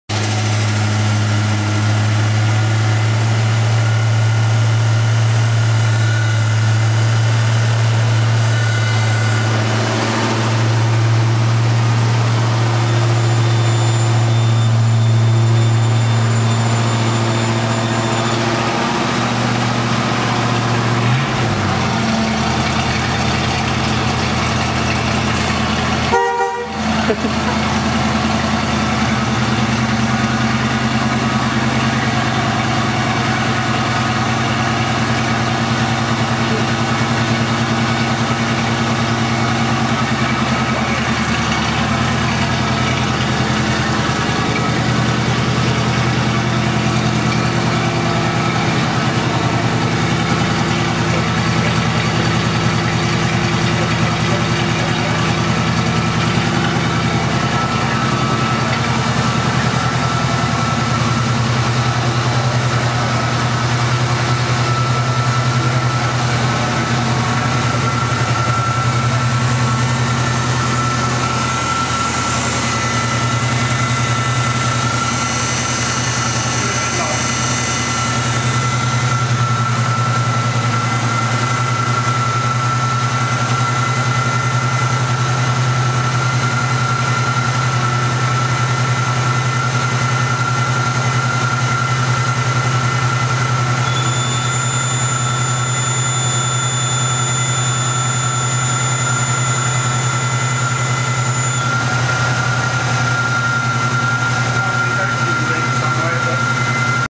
Mercury Cougar resurrection
Rich's '69 Mercury Cougar in its first week of new life. Ten years in the making, from full body-off resto basket case to immaculate gleaming beauty, this is what a life's work finally looks (and sounds) like ;0D

This is just a brief walkaround the car. Don't shoot the cameraman; yes, I suck but there ain't exactly much room in that place and you need a wide-screen lens to get the massive great thing in shot lol